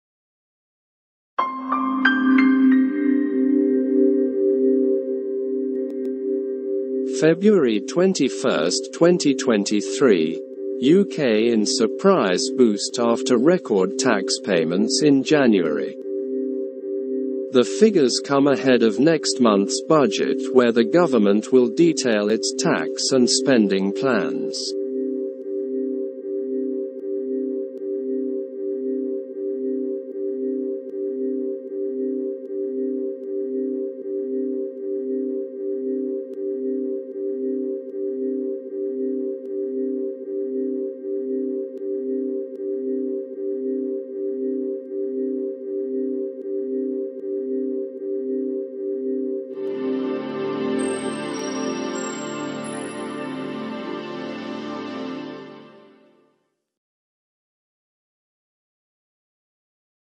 [Latest]UK in surprise boost after record tax payments in January
[Publication Date]

[Contents]
The figures come ahead of next month's Budget where the government will detail its tax and spending plans.